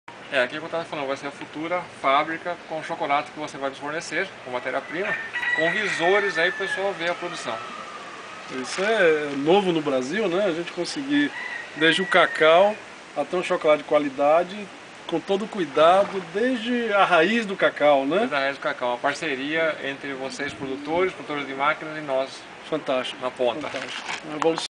Futura Fabrica de Chocolate em Itu
Proprietário da fazenda do Chocolate e produtor de cacau falam sobre a futura fábrica de chocolate em Itu.